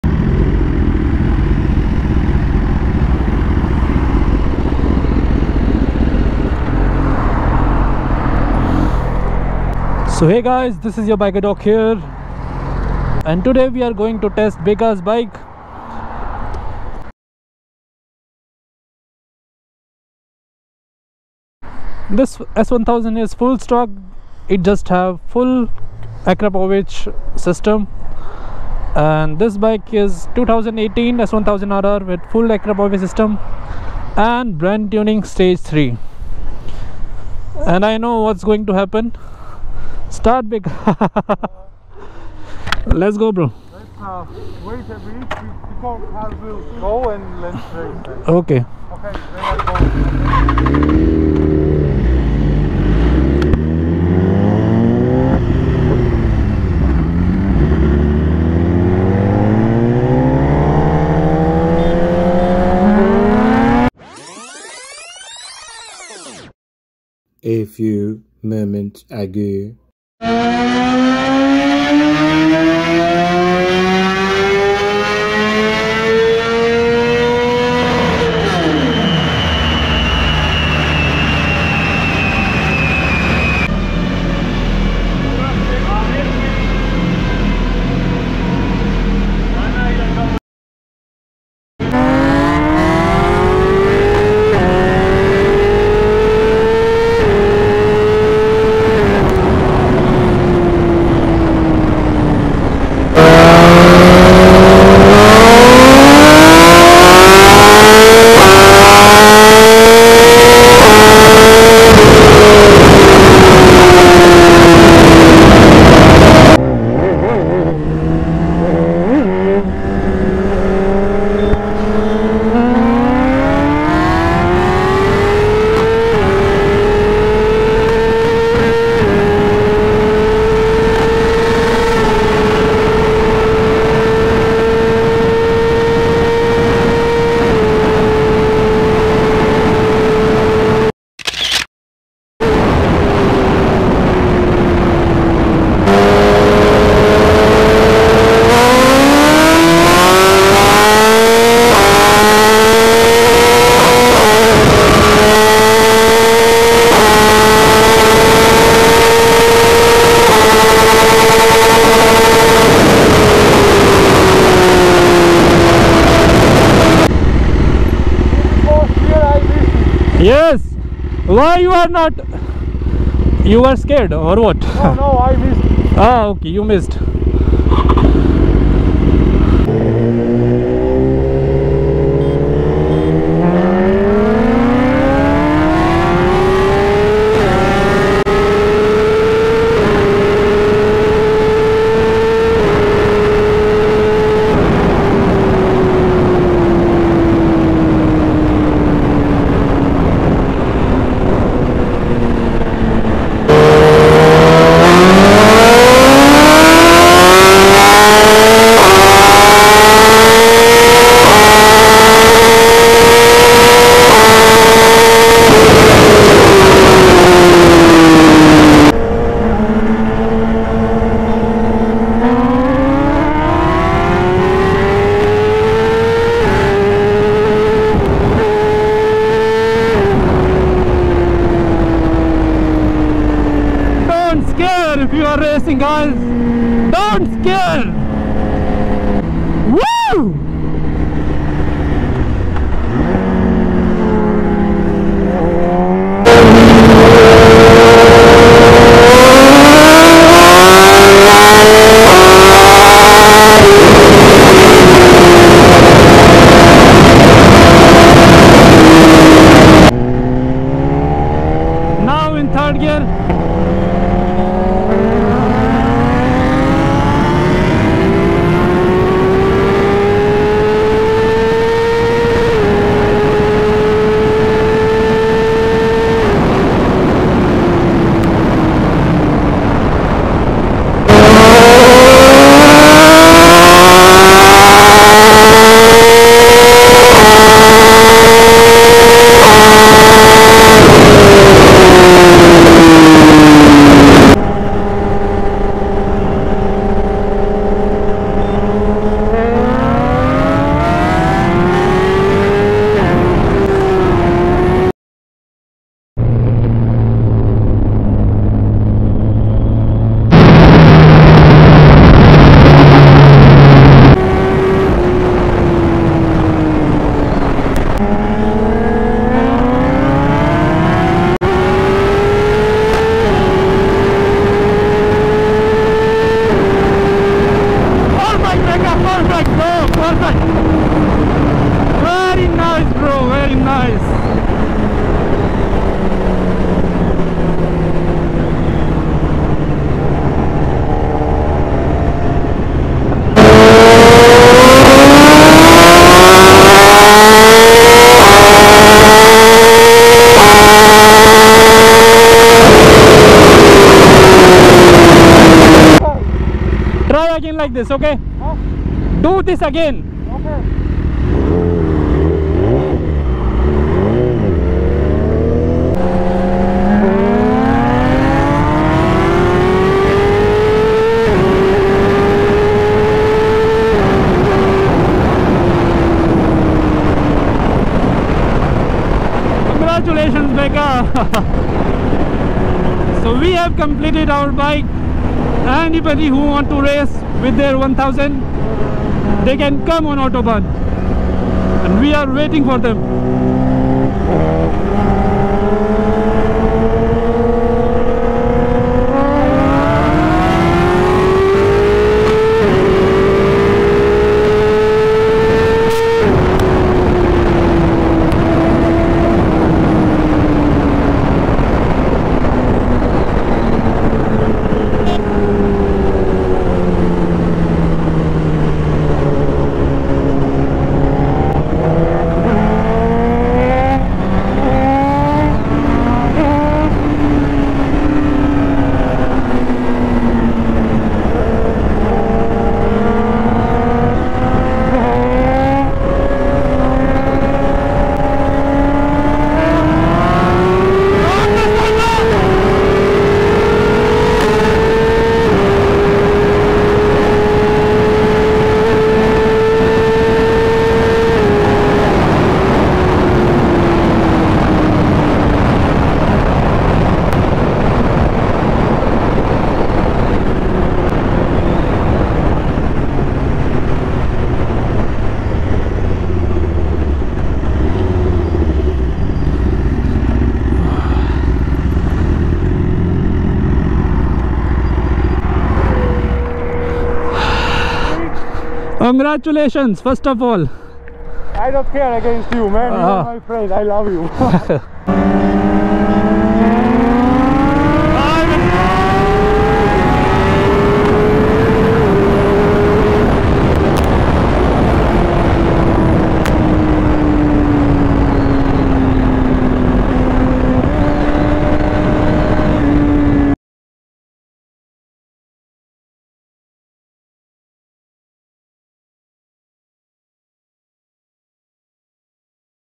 BMW S1000RR 2018 Brentuning Stage 3 VS S1000RR 2014 Stock | Is this the fastest bike in Georgia?? 😯
So my friend just got his bike tuned  and we were checking our bikes .
Please:
Comment what you want to see in future episodes

I POST COOL PHOTOS AND VIDEOS ON MY INSTAGRAM